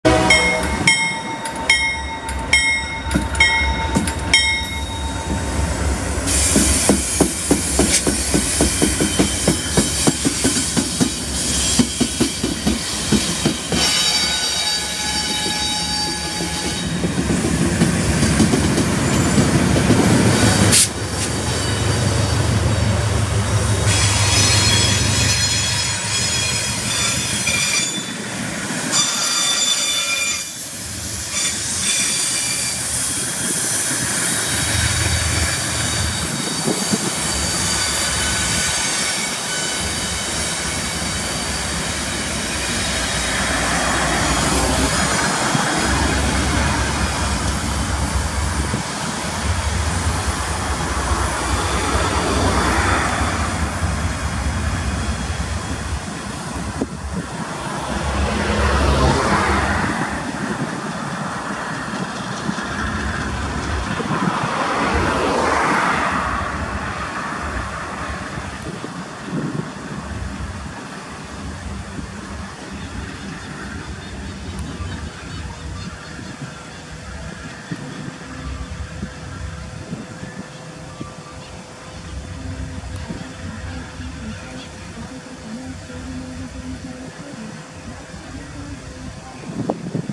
Helper de Canguera com Ac44i fibria lider
Comandando locomotiva C30-7 seguindo para engatar em trem carregado.